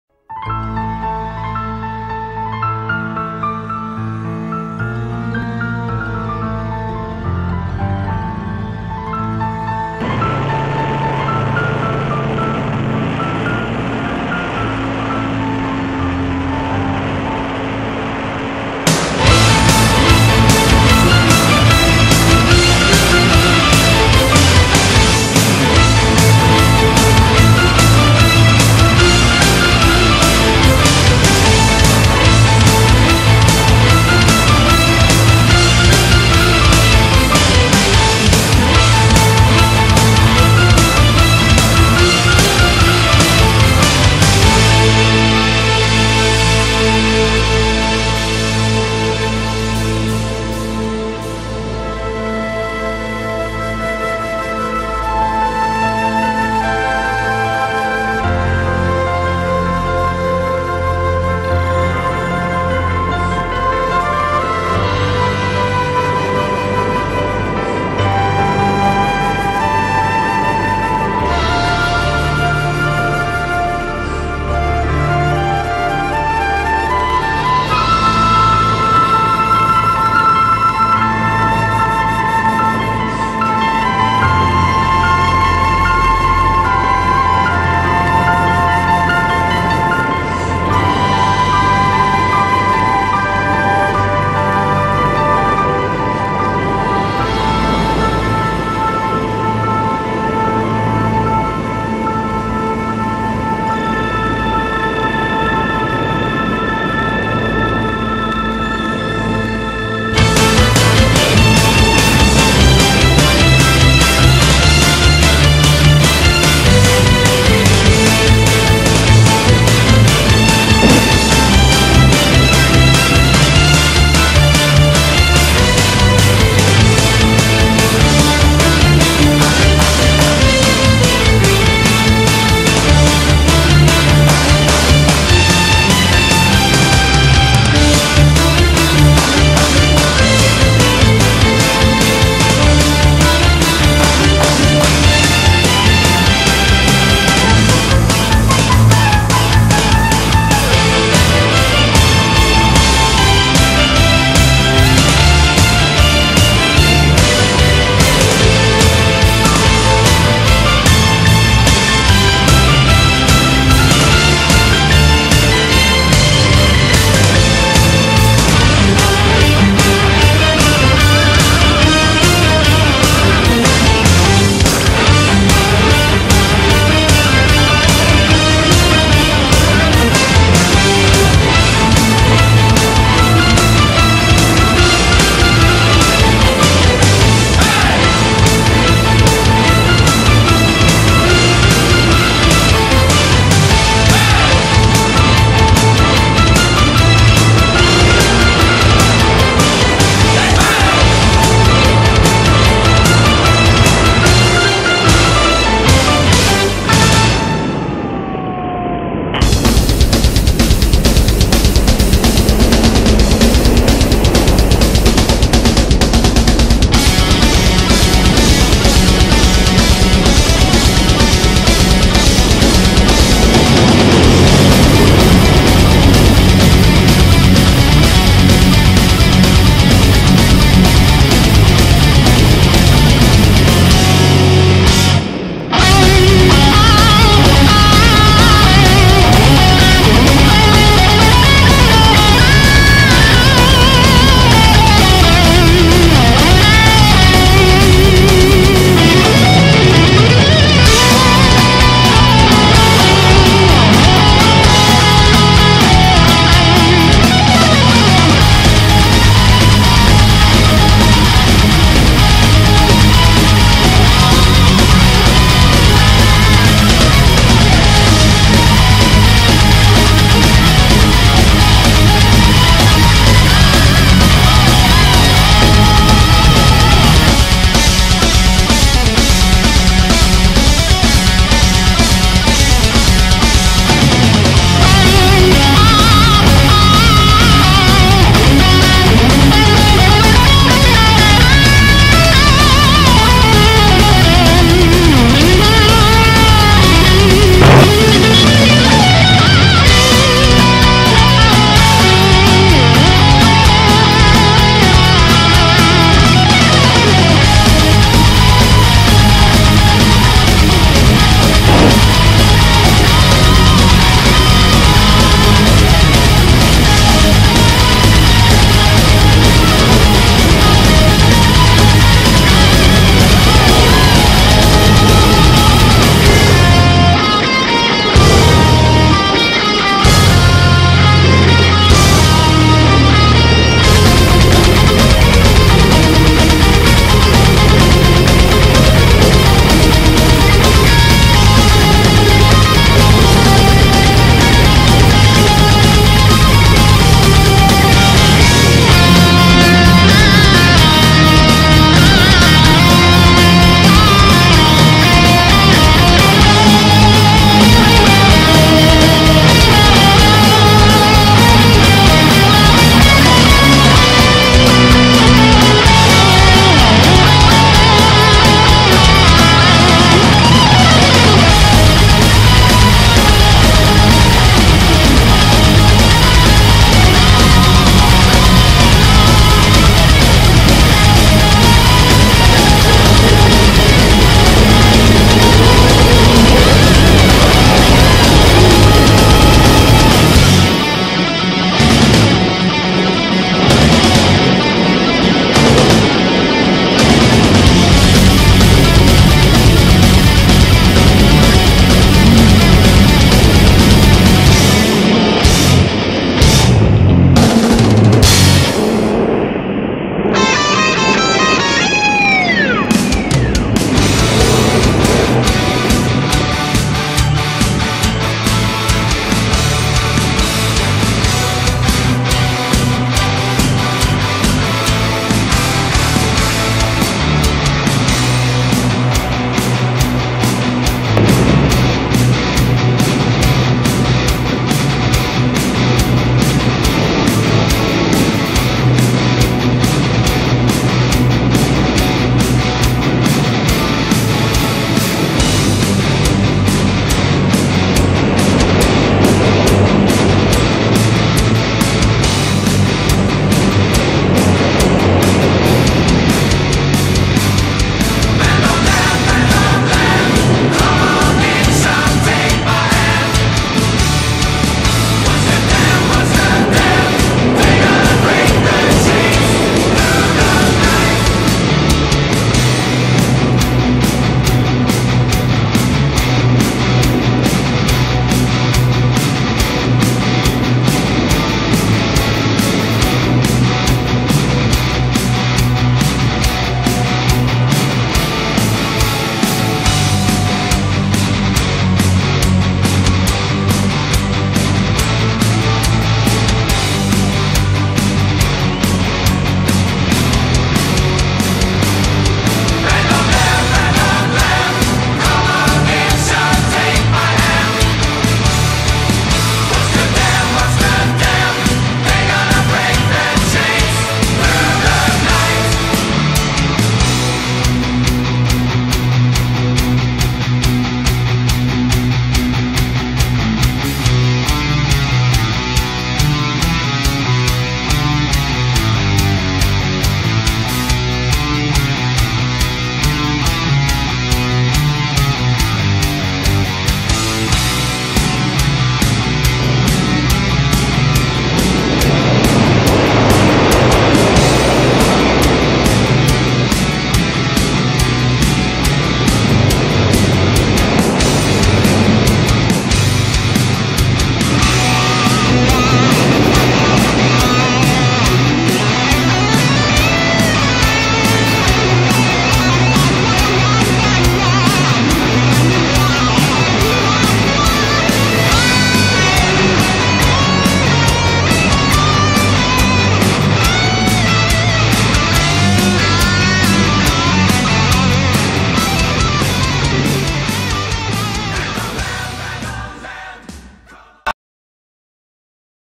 Sky Climber Flight Over Topolcianky
Recorded with HD Wing Camera 1280x720p 30fps 5MP CMOS on Sky Climber Glider.